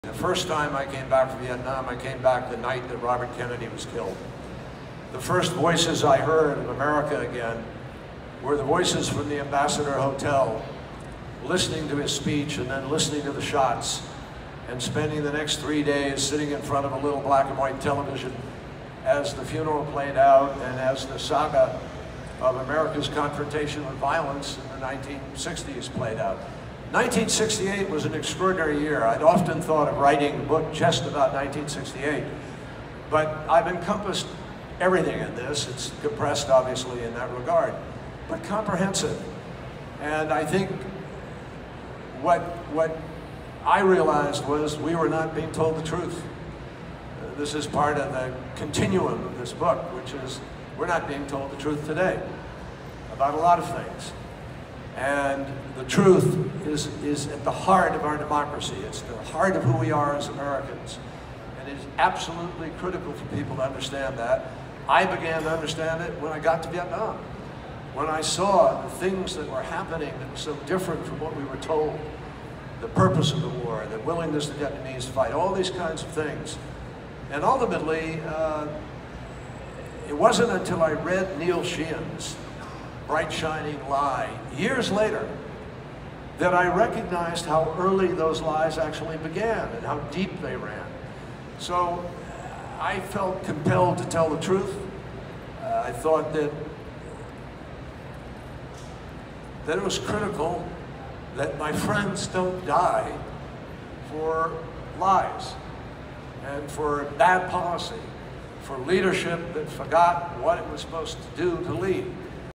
John Kerry on 1968, an extraordinary year!
...the assassination of Robert Kennedy, the failure of the American Government to tell the truth about the Vietnam War and the importance of truth to good governance in a democracy.